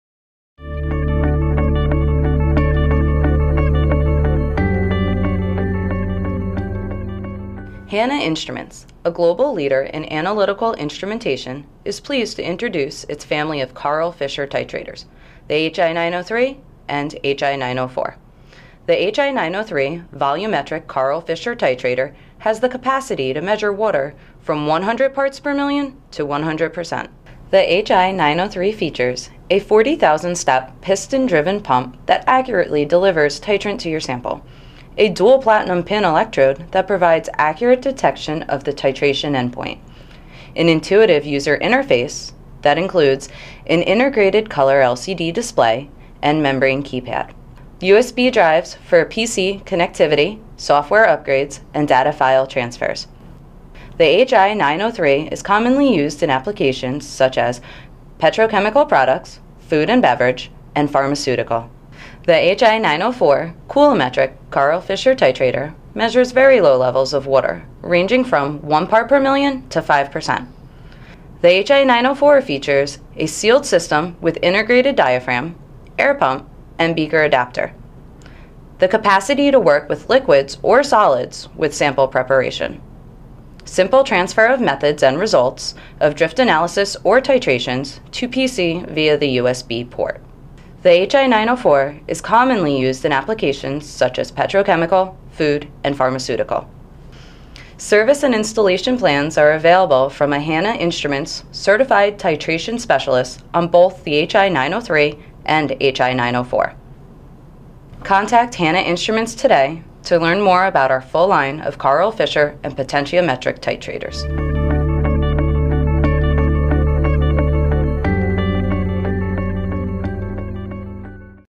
Hanna Instruments HI 903 Karl Fischer Volumetric Titrator | Instrumart
The Hanna Instruments HI 903 Karl Fischer Volumetric Titrator for moisture analysis is an extension of HANNA's highly successful potentiometric titrator platform. The HI 903 combines an ultra-high precision titrant delivery system with optically regulated magnetic stirring, sophisticated endpoint determination, dynamic dosing and background drift correction algorithms.